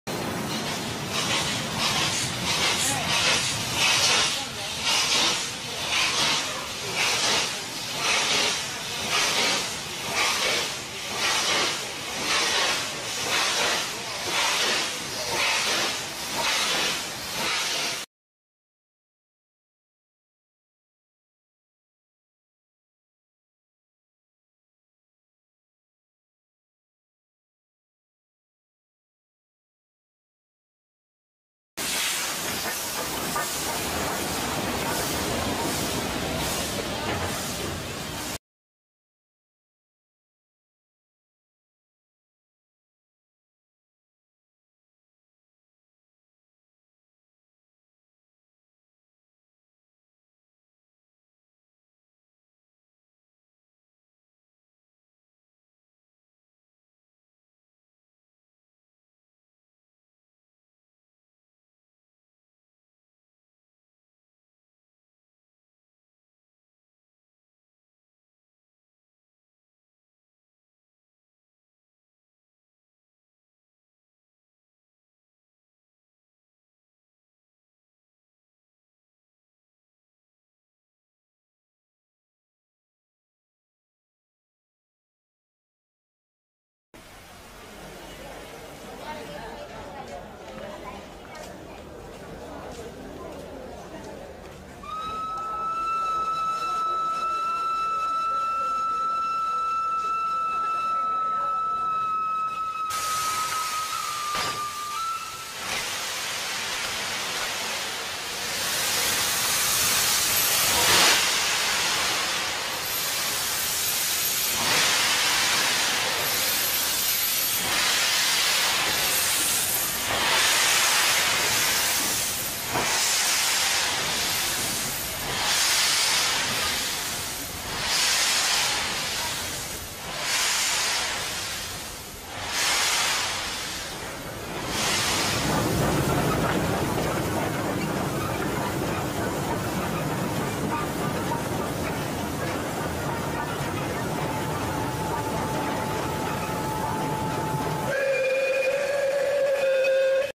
Steam and Meter Gauge era at Pondicherry (PDY)
The explicit use of the video is only for the purpose of providing information to Rail Fans about the good old days of Indian Railways.

1) HISTORY:
Pondicherry (now Puducherry) railway station (PDY) is on the Villupuram - Puducherry rail line serving the Union territory of Puducherry.

The Pondicherry railway line is one of the oldest rail link in India and it was constructed in the year 1879 by the Puducherry Railway Company during the period of French and British rule in India.

Under the supervision of South Indian Railway with the object of connecting the town and the port of Pondicherry with South India and to develop the resources of the Pondicherry Territory, the meter gauge rail link between Pondicherry and Villupuram was established

2) TRAIN:
It is the Pondicherry to Villapuram Passenger hauled by a steam loco. The train departs from Pondicherry at 1.55 PM